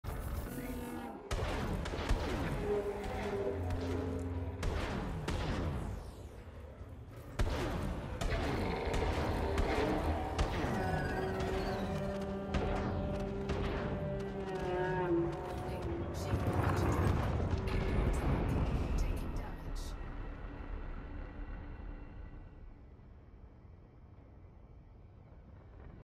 Bugging out
My mate didn't realize we'd already killed the Cyclops. Nice escape though, no caustic!